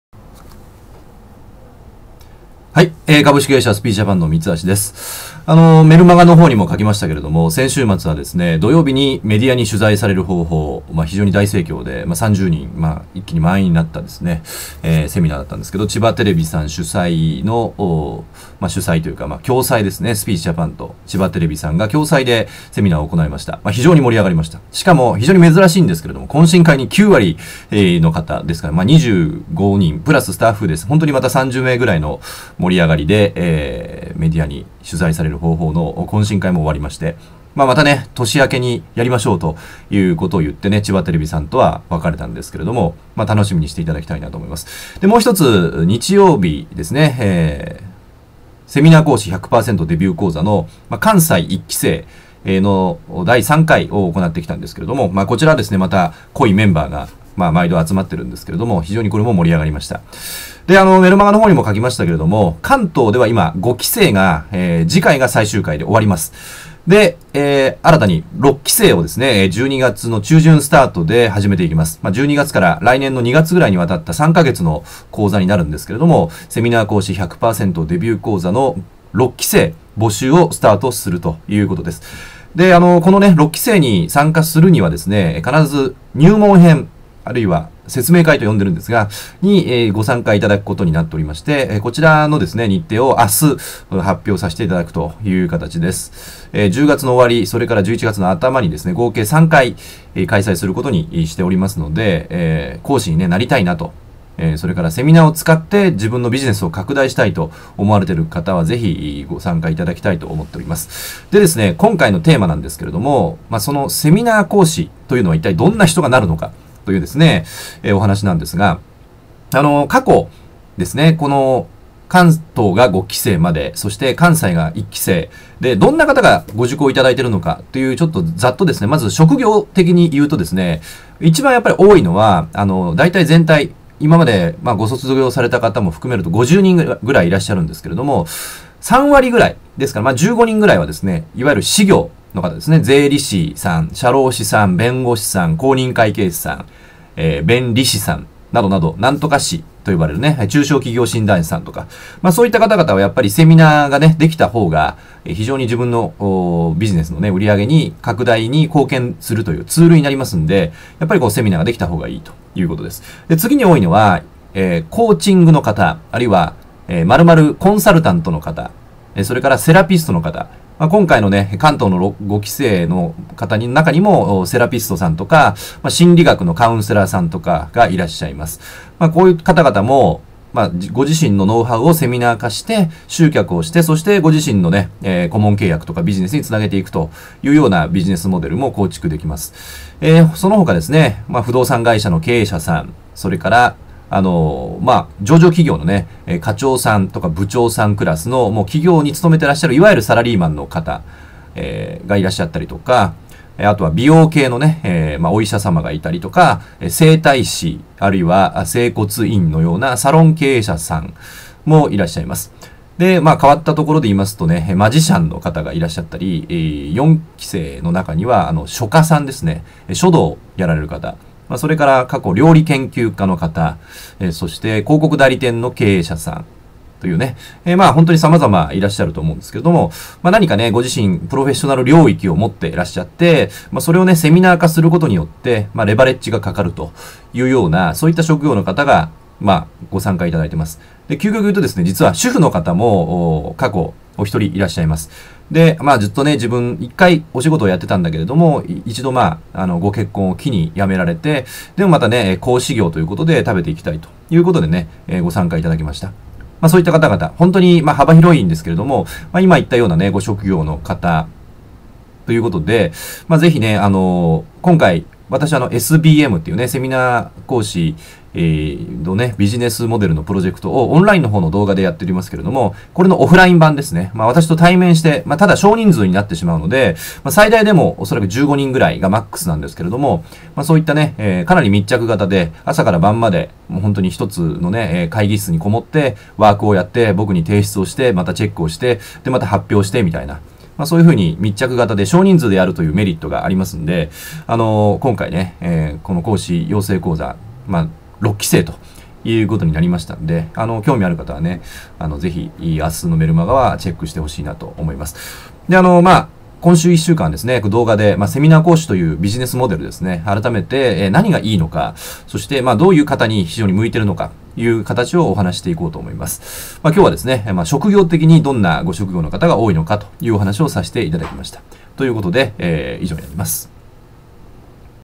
セミナー講師になるために必要な条件とは？「職業編」.wmv
株式会社スピーチジャパン代表取締役
プレゼンテーションの研修講師でもある三橋泰介が語ります。
セミナー講師１００%デビュー講座に過去どんな人が参加したのか、
そしてどうやって養成されたのかを語ります。

セミナー講師を養成する三橋泰介が語っています。

９０分のセミナー動画をプレゼント中。
あがり症克服法が・・・
株式会社スピーチジャパン